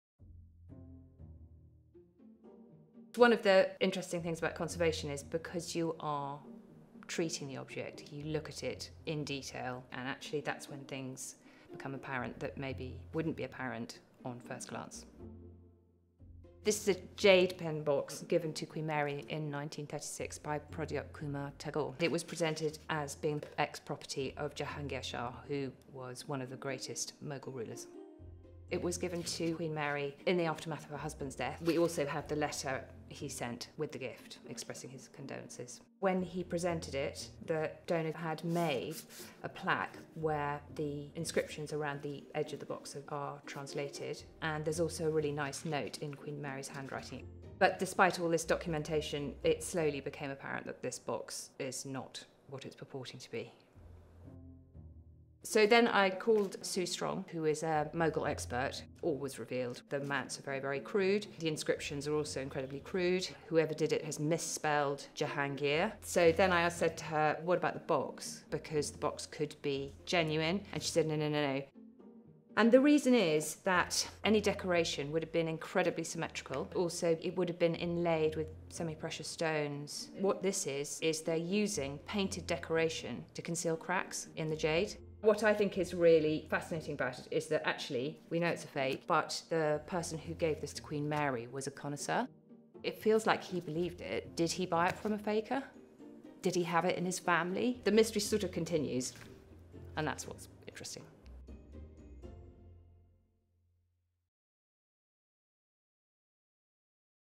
This jade box is not as it seems...
Sometimes conservation work uncovers unexpected surprises about objects in the Collection. This jade box from India was once associated with the great Mughal Emperor, Jahangir. But our conservators recently discovered there’s more to it than meets the eye.